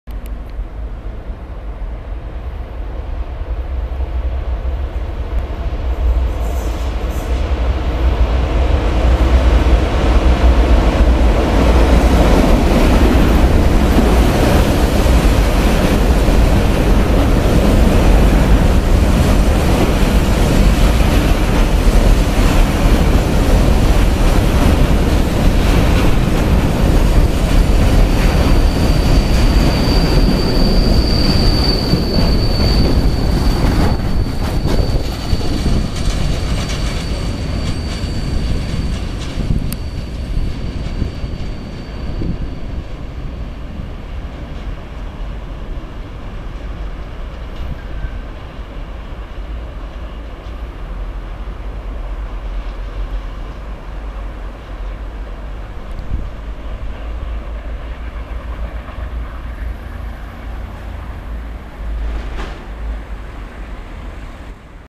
M 68132 Leandro SN-Devesas (Estação de Rio Tinto - 04Abr13)
Passagem desta composição de 11 vagões SNCF Fas Ermewa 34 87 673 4 a reboque da LD MLW 1963 - com um Sgs à cauda, carregado de "verguinha".